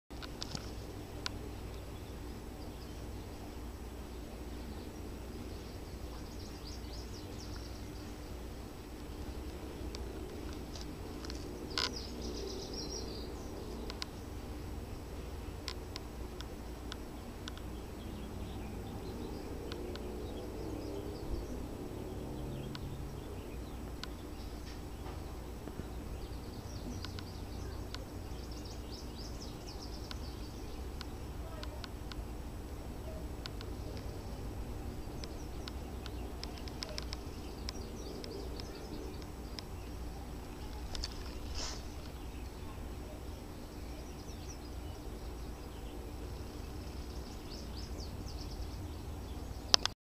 Blue headed finches (Lazuli Bunting)
These birds are migrating through Boise and they are loving the bird seed in my back yard.